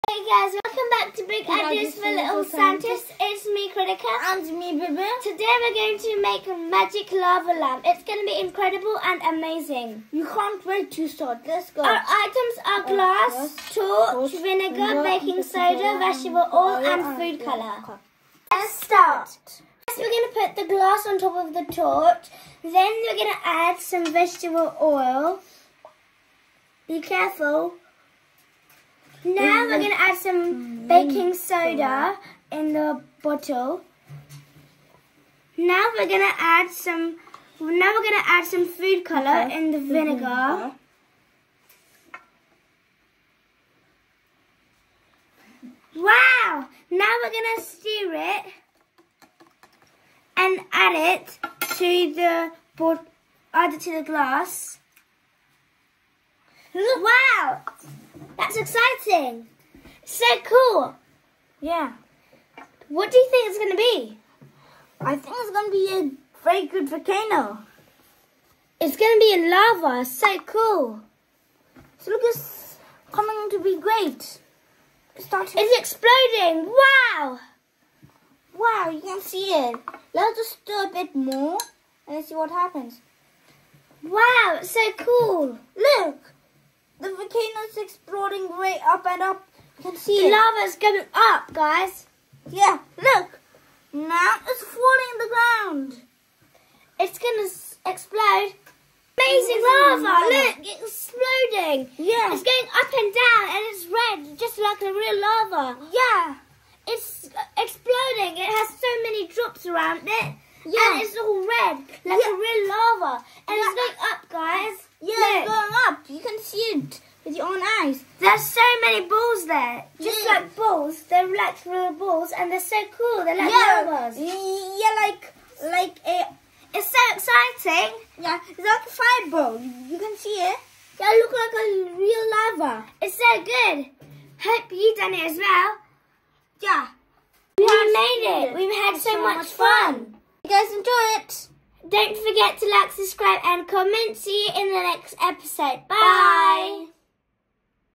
Big Ideas for Little Scientists: Magic Lava Lamp!💡
💡🌋✨ Get ready for a dazzling science adventure with Kritika and Bibhu as they reveal the secrets of making a Magic Lava Lamp! 🌋✨ Using simple household items like oil, water, food colouring, and baking soda, this experiment will create mesmerizing bubbles that dance and glow.

This fun and engaging activity is perfect for curious minds eager to explore and experiment with science. It's also a fantastic way to learn about density and chemical reactions!

📢 Materials Needed:

A clear bottle or jar

Vegetable oil

Water

Food colouring

Baking soda

Vinegar

👨‍🔬 What You'll Learn: Discover how the difference in density between oil and water, along with the reaction from baking soda and vinegar, creates a fascinating lava lamp effect!

Get ready to light up your science skills with this captivating activity! 🎉💡